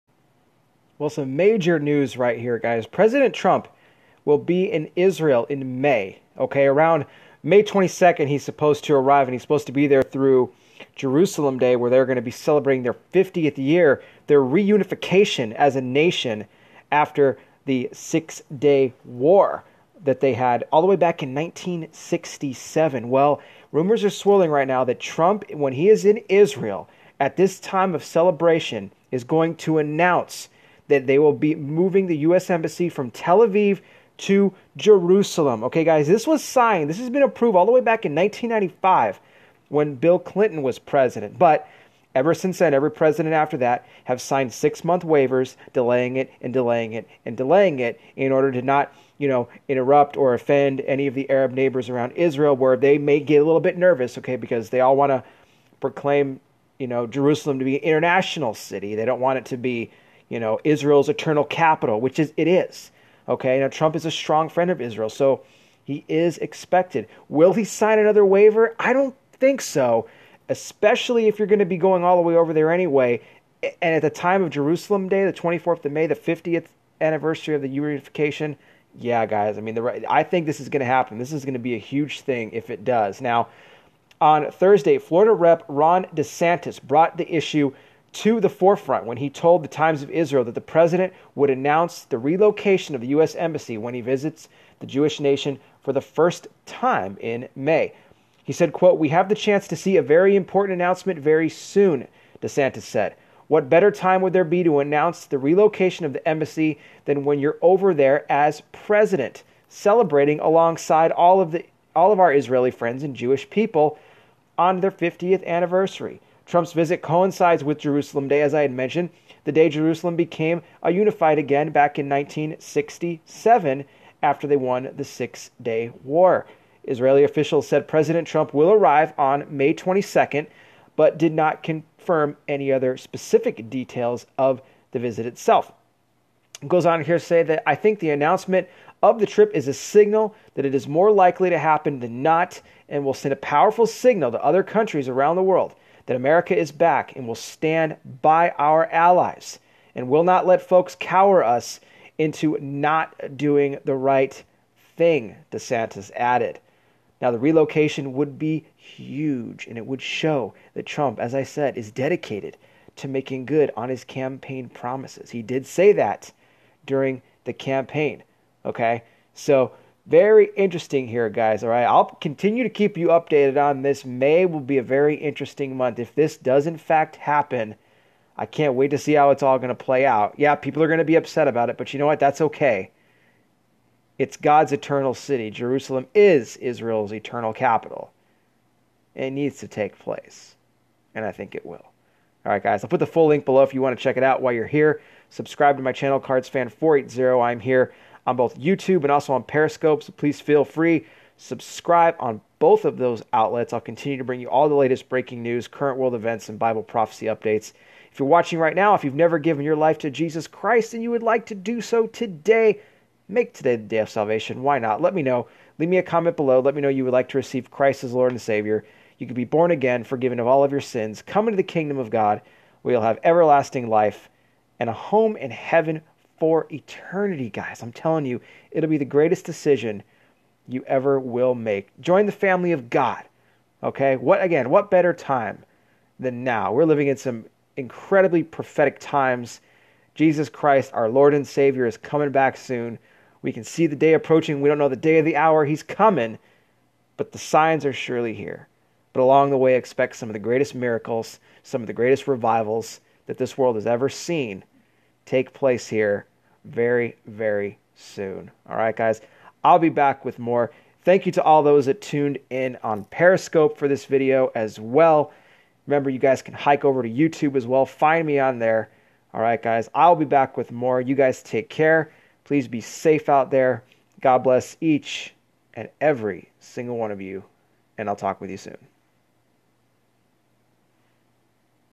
Trump To Move U.S. Embassy To Jerusalem
Trump is expected to move the U.S. Embassy from Tel Aviv to Jerusalem.